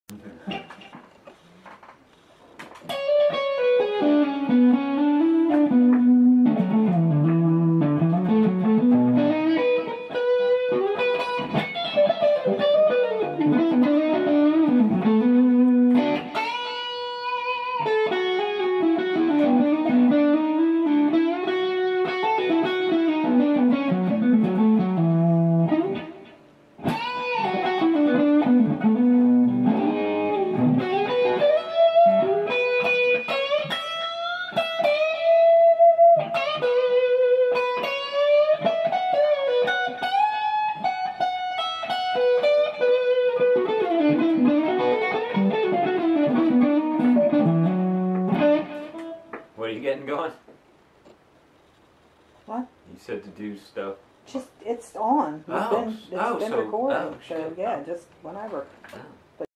Is this thing on?
The second installment of checking out the new PRS.